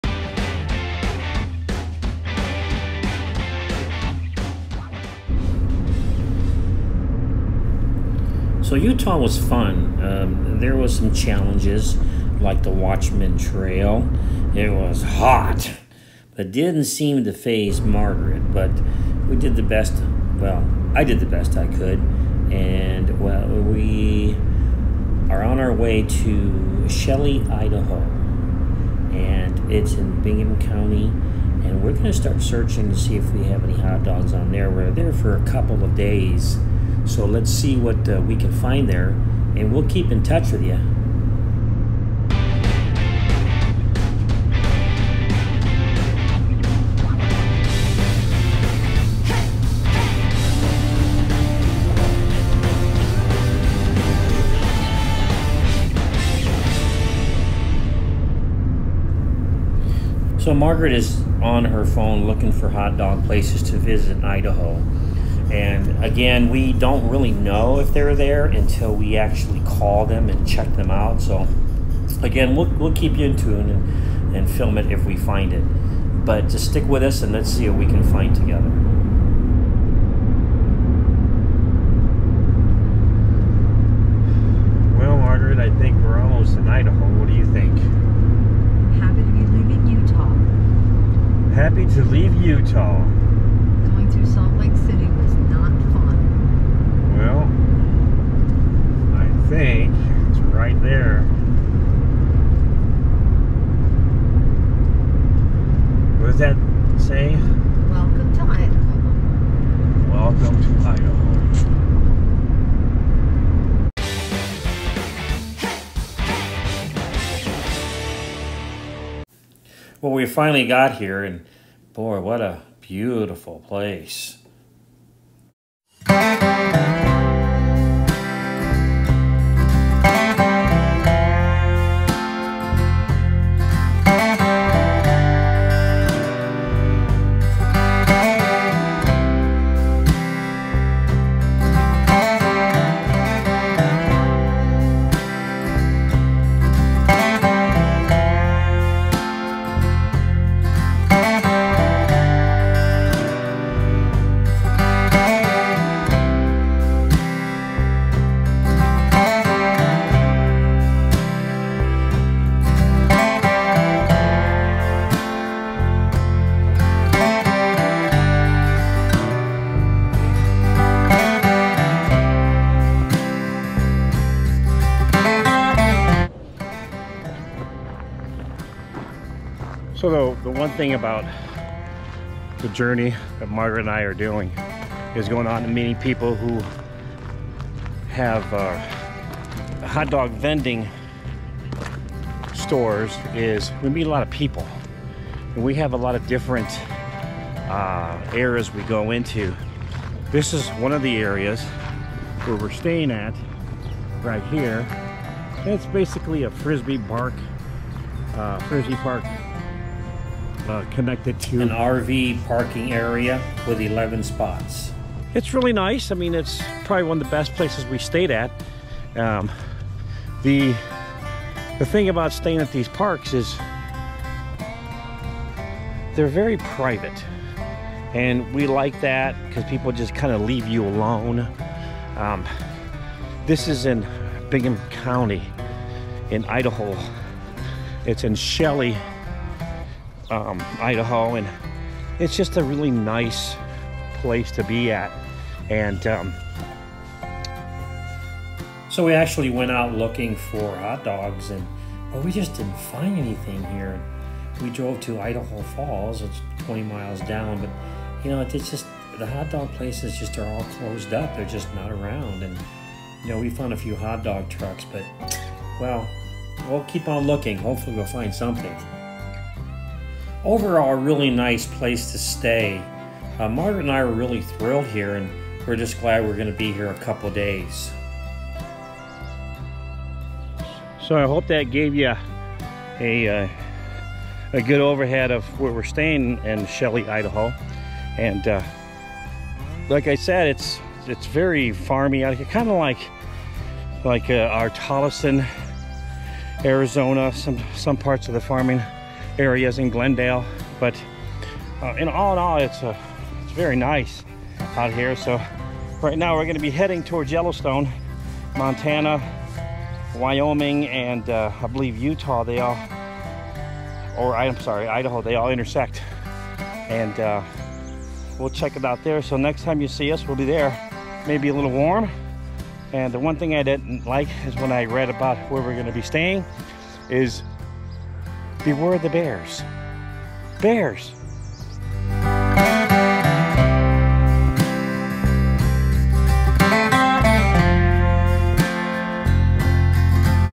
IDAHO TO YELLOWSTONE
QUICK STAY IN IDAHO ON OUR WAY TO YELLOWSTONE